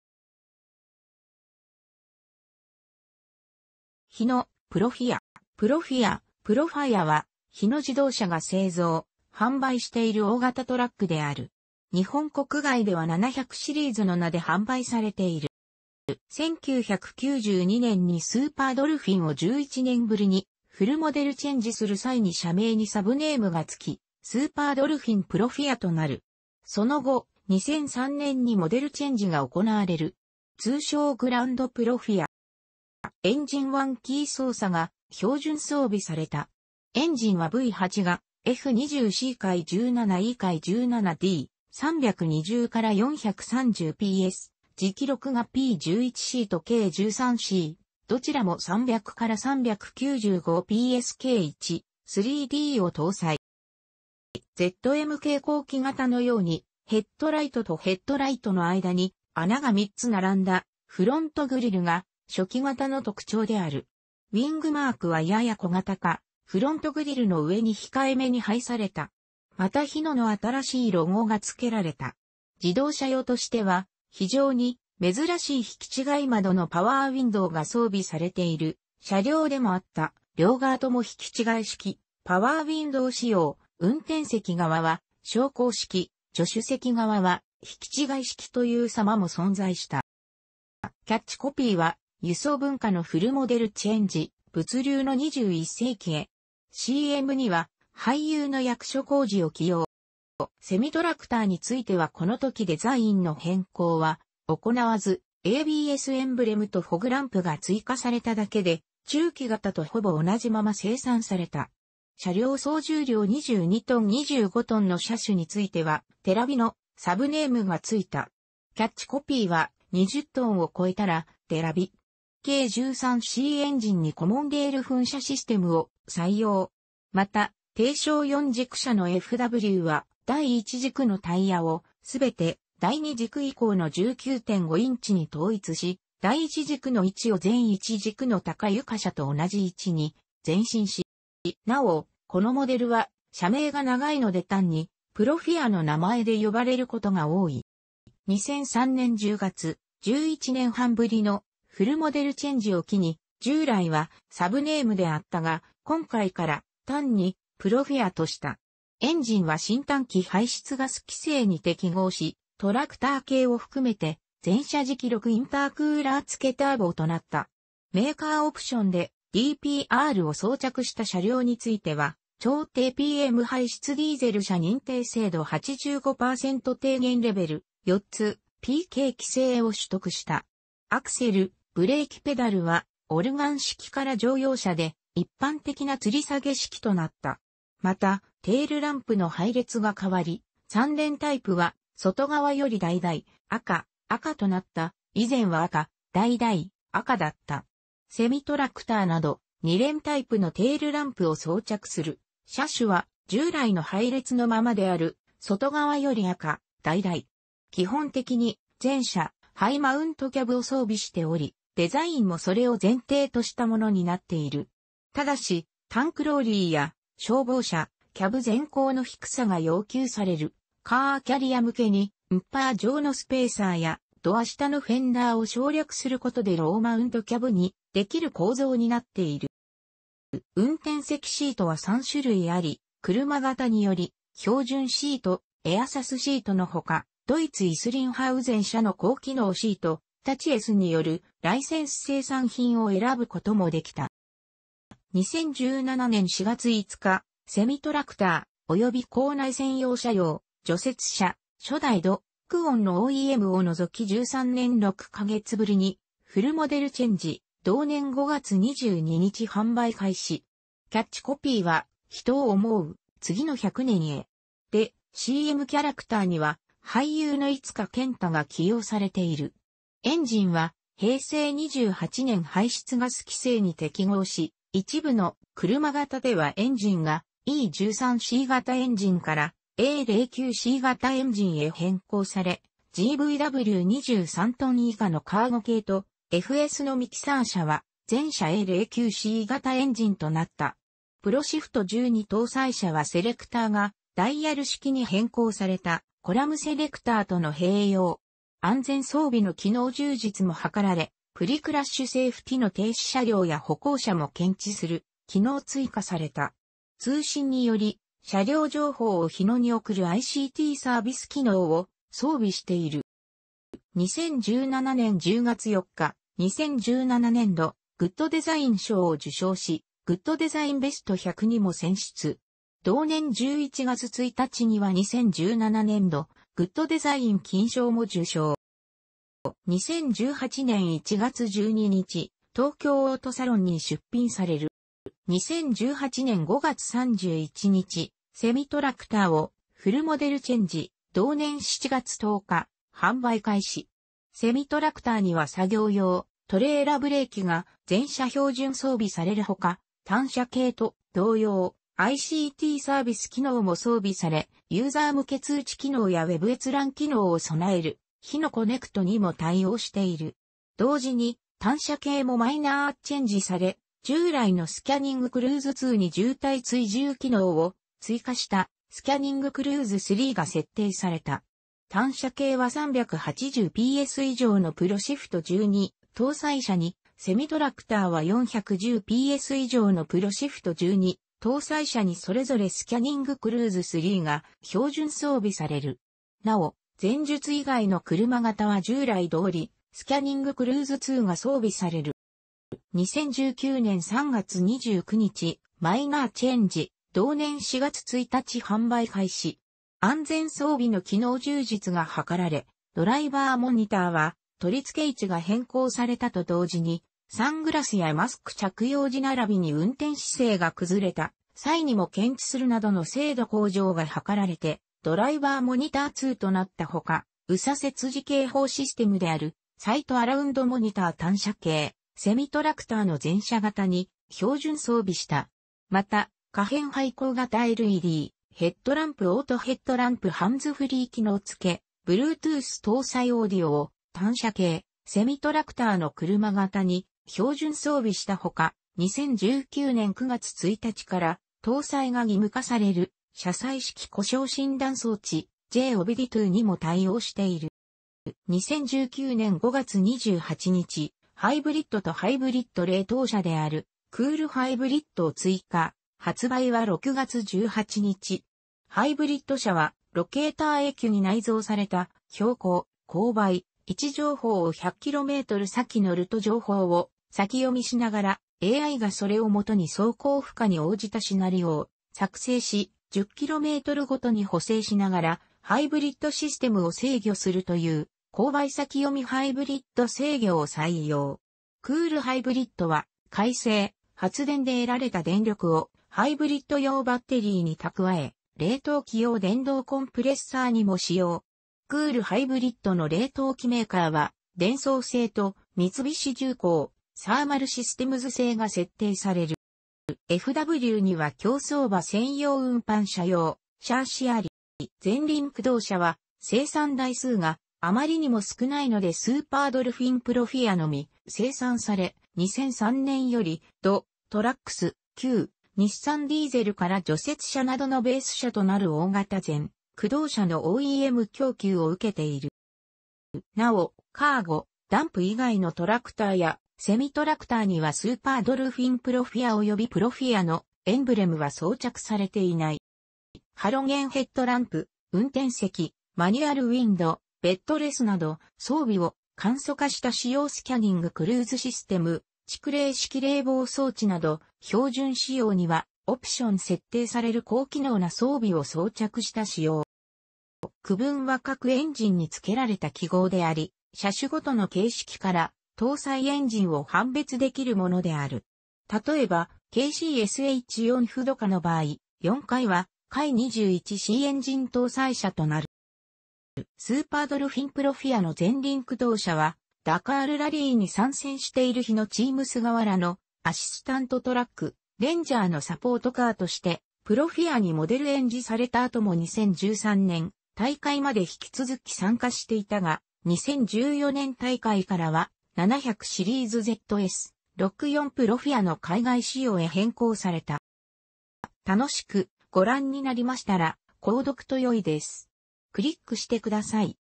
日野・プロフィア
プロフィア（"PROFIA"）は、日野自動車が製造・販売している大型トラックである｡日本国外では700シリーズの名で販売されている。
1992年にスーパードルフィンを11年ぶりにフルモデルチェンジする際に車名にサブネームが付き、スーパードルフィン・プロフィアとなる。その後、2003年にモデルチェンジが行われる。通称グランドプロフィア。

エンジンワンキー操作が標準装備された。エンジンはV8がF20C・F17E・F17D（320-430ps）、直6がP11CとK13C（どちらも300-395ps）・K13Dを搭載。

ZM系後期型のようにヘッドライトとヘッドライトの間に穴が3つ並んだフロントグリルが初期型の特徴である。ウィングマークはやや小型化、フロントグリルの上に控えめに配された。またHINOの新しいロゴが付けられた。
自動車用としては非常に珍しい引違い窓のパワーウィンドウが装備されている車両でもあった（両側とも引違い式パワーウィンドウ仕様、運転席側は昇降式・助手席側は引違い式という仕様も存在した）。

キャッチコピーは「輸送文化のフルモデルチェンジ」「物流の21世紀へ」。CMには俳優の役所広司を起用。

セミトラクターについてはこの時デザインの変更は行わず、ABSエンブレムとフォグランプが追加されただけで中期型とほぼ同じまま生産された。車両総重量22t・25tの車種については「テラヴィ」のサブネームがついた。キャッチコピーは「20tを越えたら、テラヴィ」。K13Cエンジンにコモンレール噴射システムを採用。また、低床4軸車のFWは第1軸のタイヤをすべて第2軸以降の19．5インチに統一し、第1軸の位置を前1軸の高床車と同じ位置に前進した。

なお、このモデルは車名が長いので単にプロフィアの名前で呼ばれることが多い。

2003年10月､11年半ぶりのフルモデルチェンジを機に、従来はサブネームであったが､今回から単にプロフィアとした。エンジンは新短期排出ガス規制に適合し、トラクター系を含めて全車直6インタークーラー付ターボとなった。メーカーオプションでDPRを装着した車両については超低PM排出ディーゼル車認定制度85%低減レベル（★4つ,PK-規制）を取得した。アクセル/ブレーキペダルはオルガン式から乗用車で一般的な吊り下げ式となった。また、テールランプの配列が変わり、3連タイプは外側より橙・赤・赤となった（以前は赤・橙・赤だった）。セミトラクターなど、2連タイプのテールランプを装着する車種は従来の配列のままである（外側より赤・橙）。基本的に全車ハイマウントキャブを装備しており、デザインもそれを前提としたものになっている。但し、タンクローリーや消防車、キャブ全高の低さが要求されるカーキャリア向けにバンパー上のスペーサーやドア下のフェンダーを省略することでローマウントキャブにできる構造になっている。

運転席シートは3種類あり、車型により標準シート、エアサスシートのほか、ドイツISRINGHAUSEN社の高機能シート（タチエスによるライセンス生産品）を選ぶこともできた。

2017年4月5日、セミトラクター、および構内専用車両、除雪車（初代UD・クオンのOEM）を除き13年6か月ぶりにフルモデルチェンジ（同年5月22日販売開始）。キャッチコピーは「人を思う、次の100年へ。」でCMキャラクターには俳優の猪塚健太が起用されている。エンジンは平成28年排出ガス規制に適合し、一部の車型ではエンジンがE13C型エンジンからA09C型エンジンへ変更され、GVW23t以下のカーゴ系と、FSのミキサー車は全車A09C型エンジンとなった。Proshift12搭載車はセレクターがダイヤル式に変更された（コラムセレクターとの併用）。安全装備の機能充実も図られ、プリクラッシュセーフティの停止車両や歩行者も検知する機能も追加された。通信により車両情報を日野に送るICTサービス機能を装備している。

2017年10月4日、2017年度グッドデザイン賞を受賞し、グッドデザイン・ベスト100にも選出。同年11月1日には2017年度グッドデザイン金賞も受賞。

2018年1月12日、東京オートサロンに出品される。

2018年5月31日、セミトラクターをフルモデルチェンジ（同年7月10日販売開始）。セミトラクターには作業用トレーラブレーキが全車標準装備される他、単車系と同様、ICTサービス...